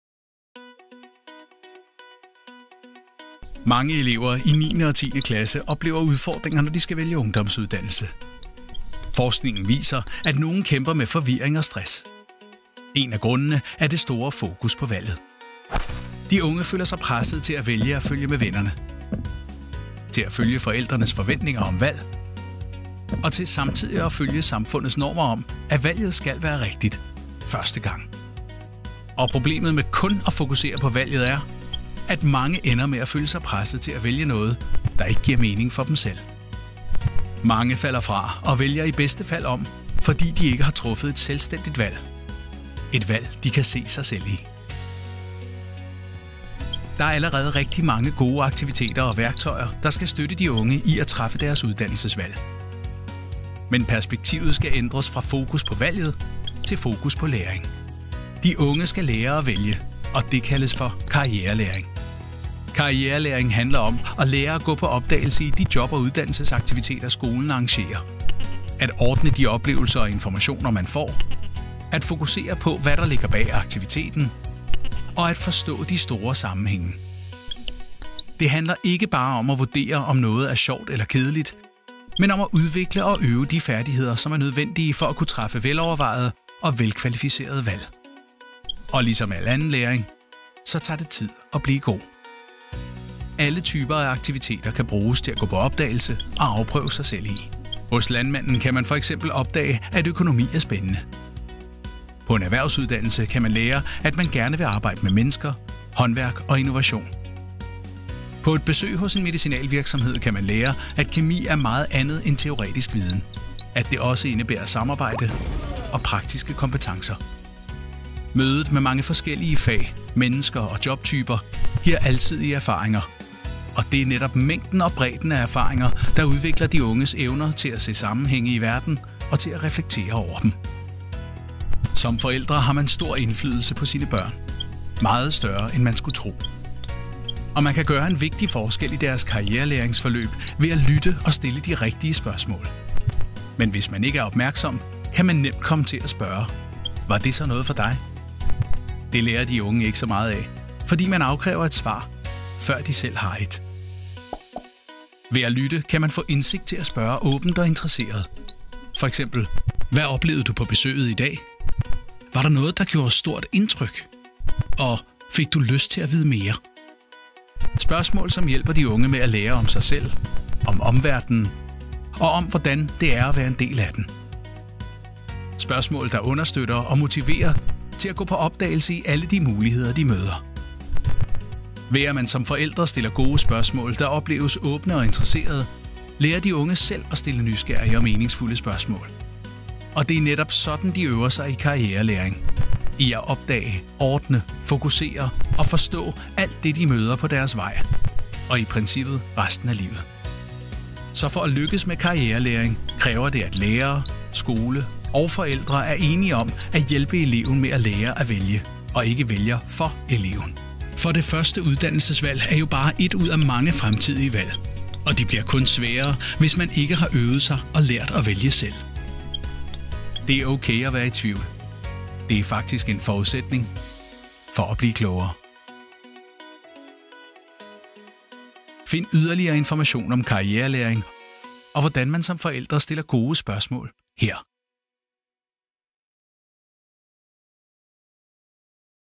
Udsyn i udskolingen (forældre) - Whiteboard film by Bulldog and Partners
KL (Local Government Denmark) and DLF (The Danish Union of Teachers) had just finished a large research project and needed to get the results out to their target audience. So Bulldog and Partners were asked to help with the film production – from cultivating the background material, building the storyline and visual concept to the final production.

The research project focused on improving school children’s decisions when choosing which education or carer they wanted to pursue. As it where, too many was dropping out or switching track – causing stress and other difficulties.

The research project found, that a fundamental shift was needed. Instead of asking the kids “what they wanted to do”, parents and teachers should keep all options open and help the kids learn how to choose – not what to choose.

We created 3 films, each aimed at a specific audience: teachers, parents and the school administrations. The films were produced in our whiteboard format to maximize understanding and provide each audience with a unique perspective on the subject.

All 3 films take you on a journey with the kids. You see what they see. Experience how they learn. And understand why you need to change your approach – in order to help them make the decisions that are right for them.